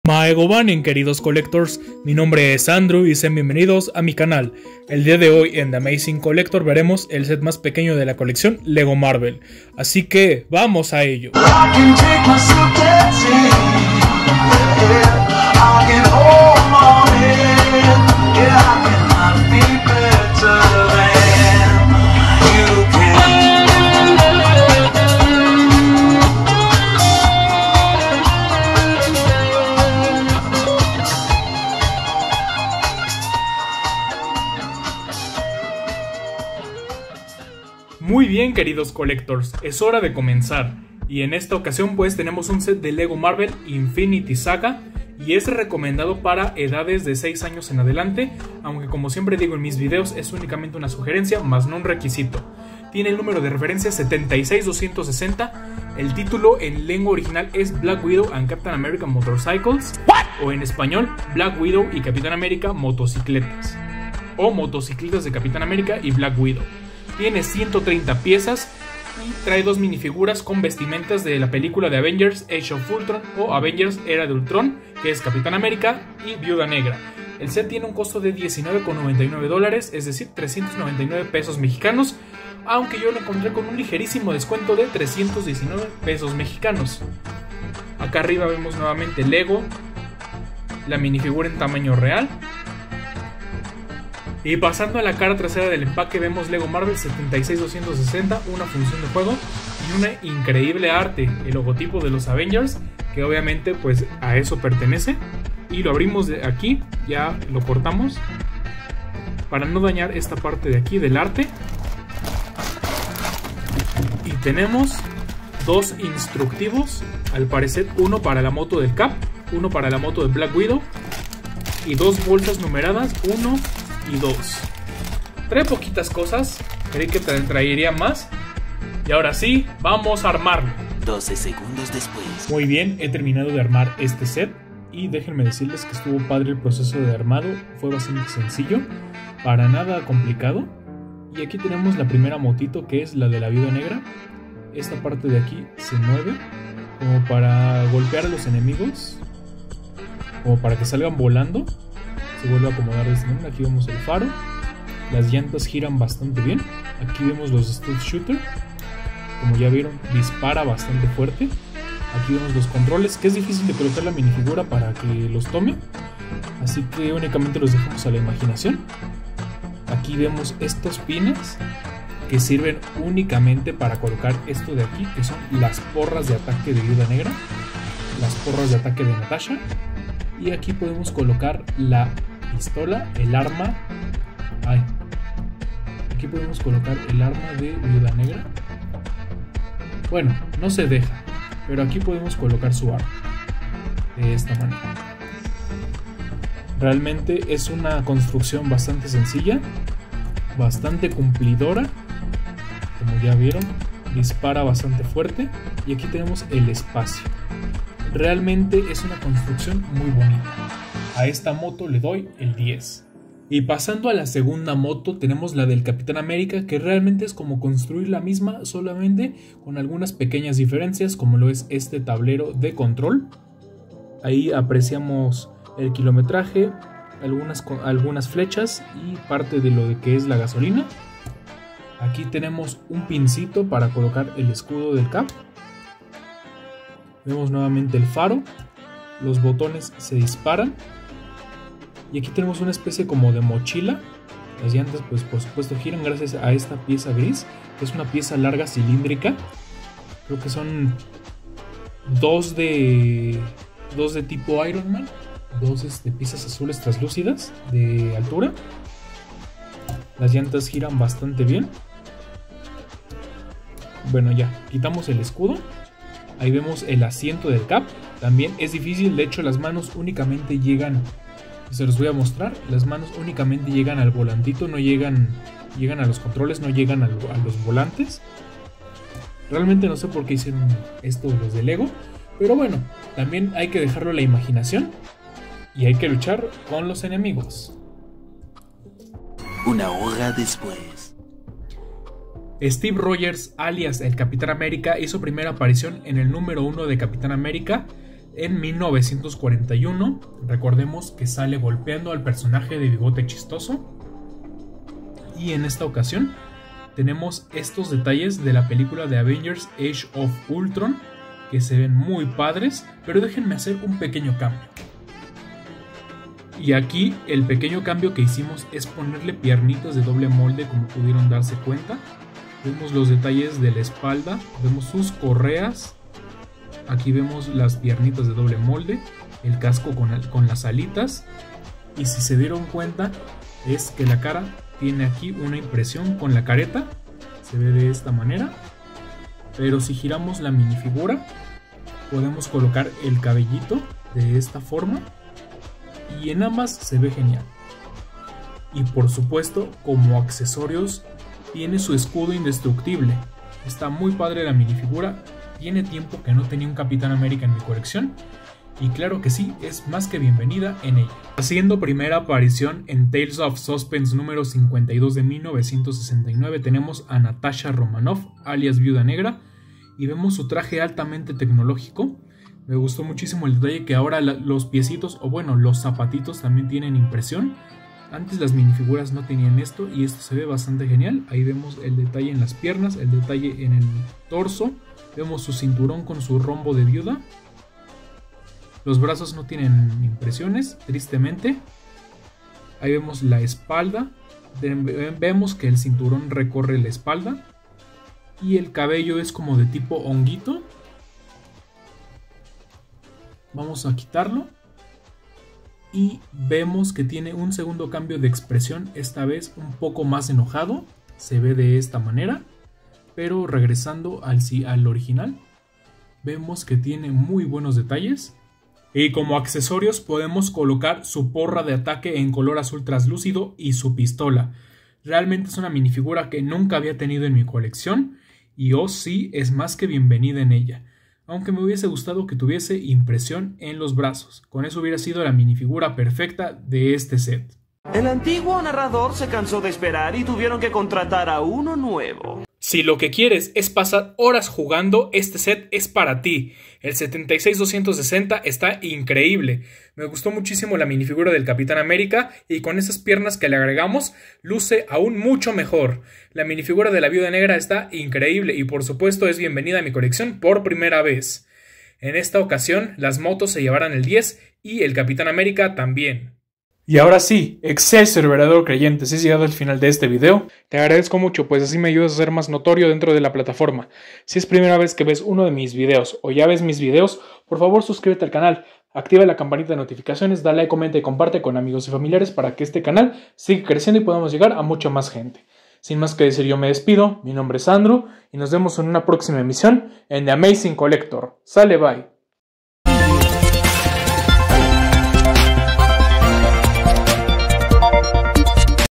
Motos de Black Widow y Capitán América (76260)
Construye las impresionantes motos que Black Widow y el Capitán América utilizaron para surcar el campo de batalla y enfrentarse a las fuerzas de la malvada Hydra.

¡Prepárate para vivir emocionantes batallas y crear tus propias historias de superhéroes con el set LEGO Marvel #76260 Motos de Black Widow y Capitán América! ¡La aventura te espera en cada giro de rueda!